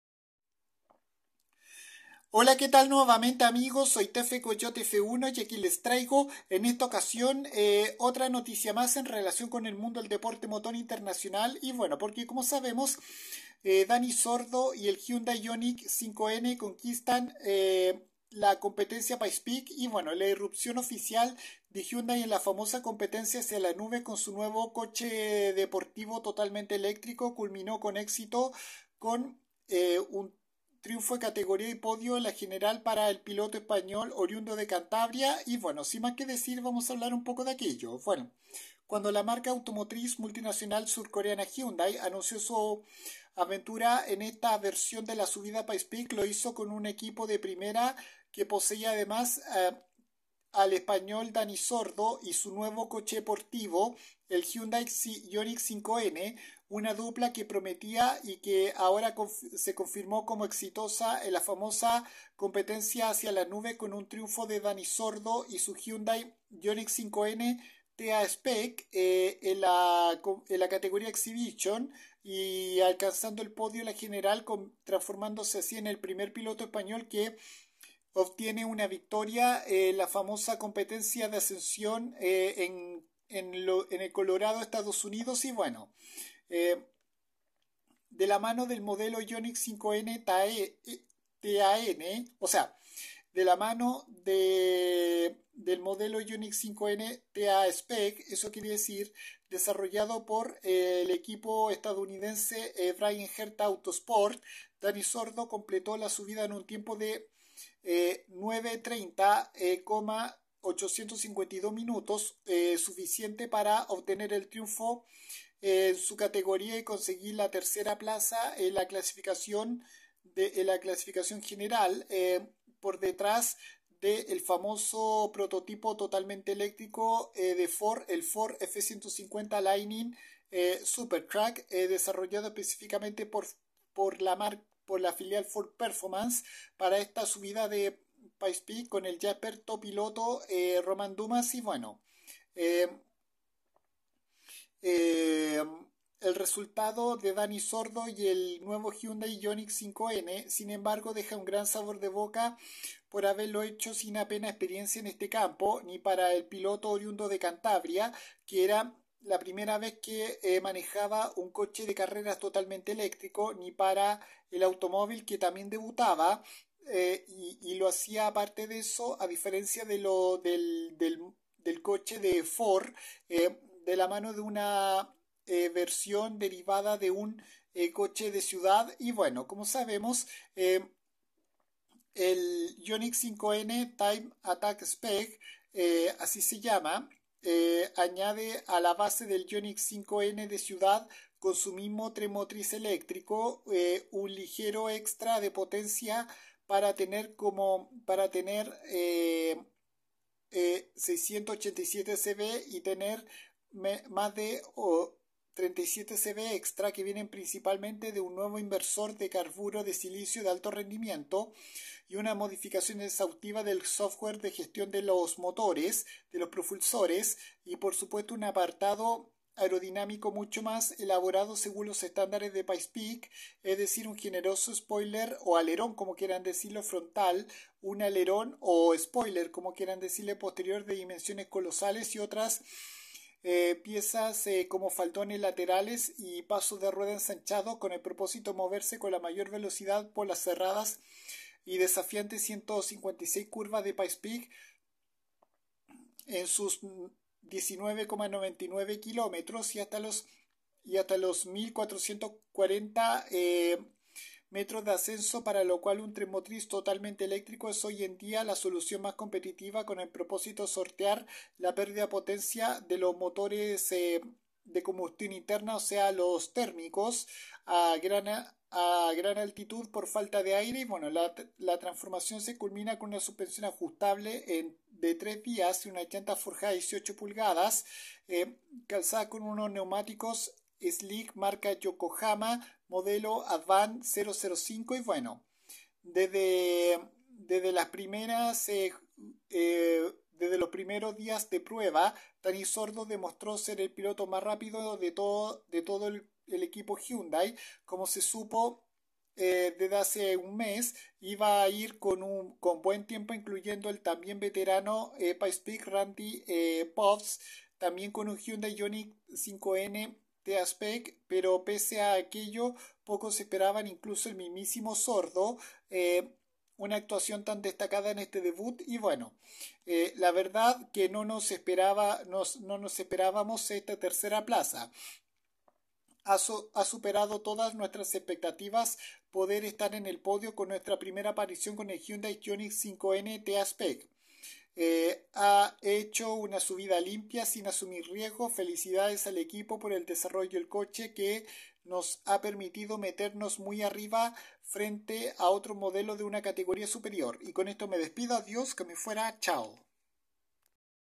Dani Sordo conquista Pikes Peak con el Hyundai Ioniq 5 N
Era la 1era vez del piloto español oriundo de Cantabria en esta famosa competencia de la subida a esta montaña de EEUU y con este éxito logró el 3er puesto en la general